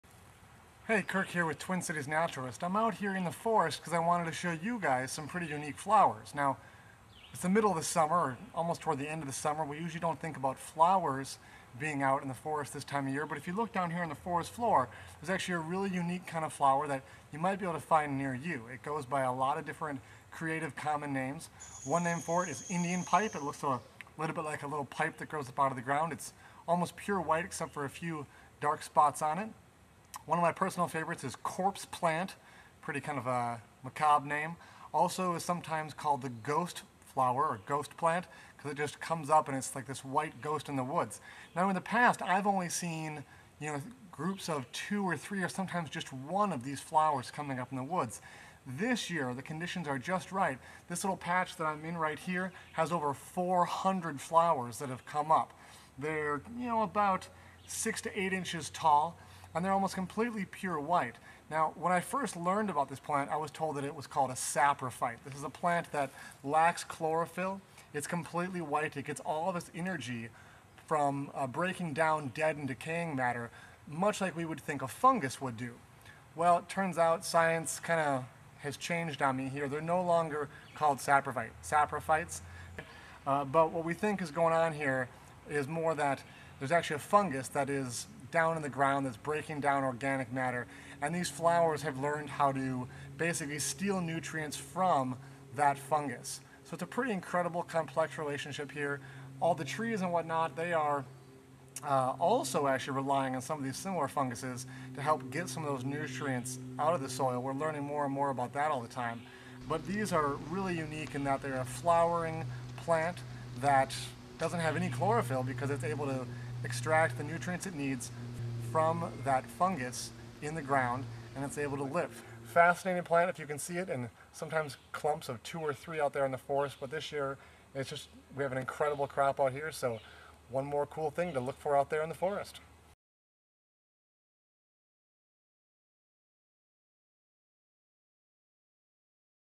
Indian Pipe in the Forest
Myco-heterozygotes in the woods! Conditions were just right this year for hundreds of Indian Pipe to come up in the Minnesota woods.